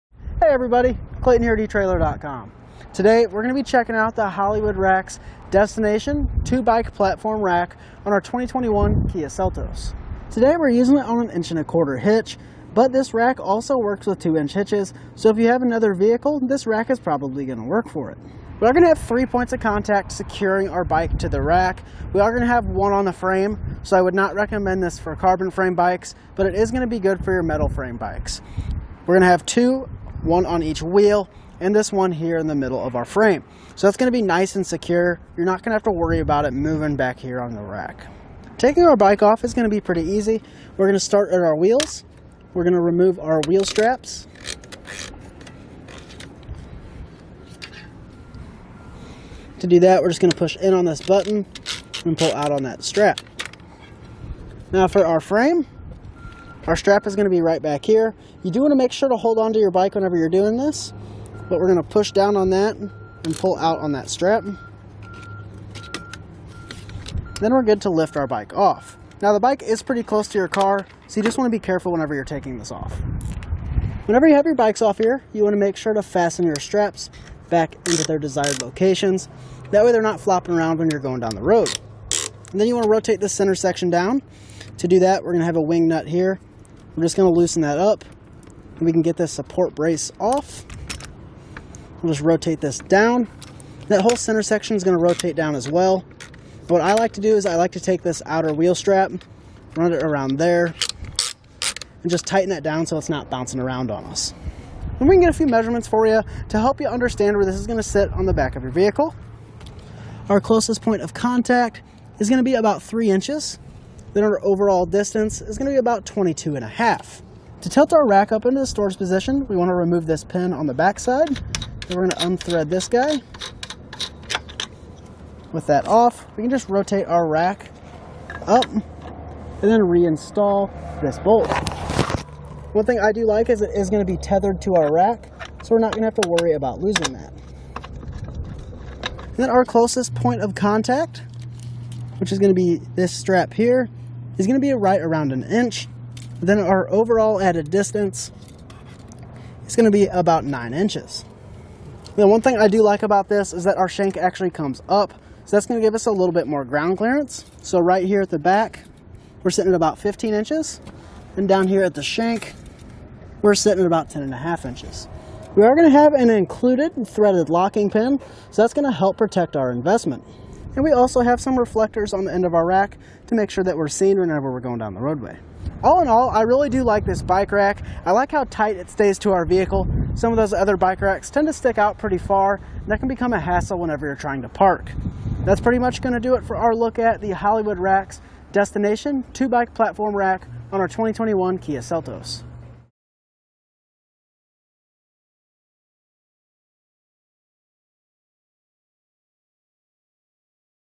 etrailer | Hollywood Racks Hitch Bike Racks Review - 2021 Kia Seltos
Hi, everybody Clayton here etrailer.com. Today, We're gonna be checking out the Hollywood racks destination two bike platform rack on our 2021 Kia Seltos. Today, we're using it on an inch and a quarter hitch but this rack also works with two inch hitches. So, if you have another vehicle this rack is probably gonna work for it. We gonna have three points of contact securing our bike to the rack. We are gonna have one on the frame.

So, I would not recommend this for carbon frame bikes but it is going to be good for your metal frame bikes. We're going to have two, one on each wheel and this one here in the middle of our frame. So, it's going to be nice and secure. You're not gonna have to worry about it, moving back here on the rack. Taking your bike off it's gonna be pretty easy.

We're gonna start at our wheels. We're gonna remove our wheel straps. To do that, We're just going to push in on this button and pull out on that strap. Now for our frame, our strap is going to be right back here. You do wanna make sure to hold onto your bike whenever you're doing this but we're going to push down on that and pull out on that strap.

Then, We're good to lift our bike off. Now the bike is pretty close to your car. So, you just want to be careful whenever you're taking this off. Whenever you have your bikes off here you want to make sure to fasten your straps back into their desired locations. That way, they're not flopping around when you're going down the road.

And then, you want to rotate this intersection down. To do that, We're going to have a wing nut here. I'm just gonna loosen that up and we can get the support brace off. Just rotate this down. That whole center section is going to rotate down as well. But, I like to do is, I like to take this outer wheel strap run it around there and just tighten that down. So it's not bouncing around on us. We can get a few measurements for you to help you understand where this is going to sit on the back of your vehicle. Our closest point of contact is going to be about three inches. Then, Our overall distance is gonna be about 22 and a half. To tilt our rack up in the storage position, We want to remove this pin on the back side. Then, We're gonna unthread this guy with that off. We can just rotate our rack up. Re-install this bolt. One thing I do like is going to be tethered to our rack. So, We're not gonna have to worry about losing that. Then, Our closest point of contact which is going to be this strap here is going to be a right around an inch. Then, Our overall added distance it's going to be about nine inches. The one thing I do like about this is that our shank actually comes up. So, that's going to give us a little bit more ground clearance. So right here at the back, we're sitting at about 15 inches and down here at the shank, we're sitting at about 10 and a half inches. We are going to have an included and threaded locking pin. So, that's going to help protect our investment. We also have some reflectors on the end of our rack to make sure that we're seen whenever we're going down the roadway. All in all, I really do like this bike rack. I like how tight it to our vehicle. Some of those other bike racks tend to stick out pretty far and that can become a hassle. Whenever you're trying to park. That's pretty much going to do it for our look at the Hollywood racks destination two bike platform rack on our 2021 Kia Santos..